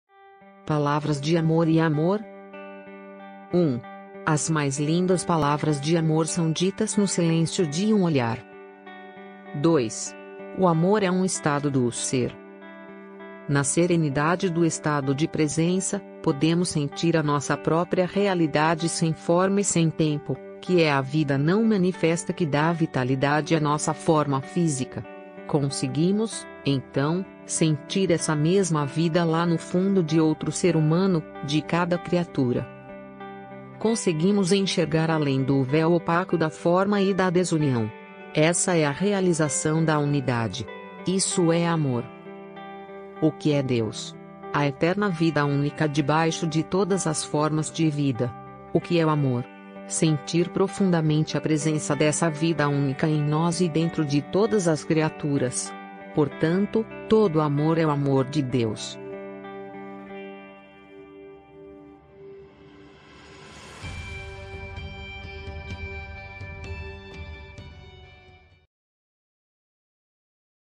Palavras de Amor & Amor
1. As mais lindas PALAVRAS DE AMOR são ditas no SILÊNCIO DE UM OLHAR.

2. O amor é um estado do Ser.

Na serenidade do estado de presença, podemos sentir a nossa própria realidade sem forma e sem tempo, que é a vida não manifesta que dá vitalidade à nossa forma física. Conseguimos, então, sentir essa mesma vida lá no fundo de outro ser humano, de cada criatura.

Conseguimos enxergar além do véu opaco da forma e da desunião. Essa é a realização da unidade. Isso é amor.

O que é Deus? A eterna Vida Única debaixo de todas as formas de vida. O que é o amor? Sentir profundamente a presença dessa Vida Única em nós e dentro de todas as criaturas. Portanto, todo amor é o amor de Deus.

Fonte:

1. Leonardo da Vinci. — Frases de Sabedoria: Citações de importantes figuras de nossa história e cultura para você refletir e evoluir como ser humano de Alisson Junior.

2. Tolle, Eckhart. O poder do agora: Um guia para a iluminação espiritual.

Namastê.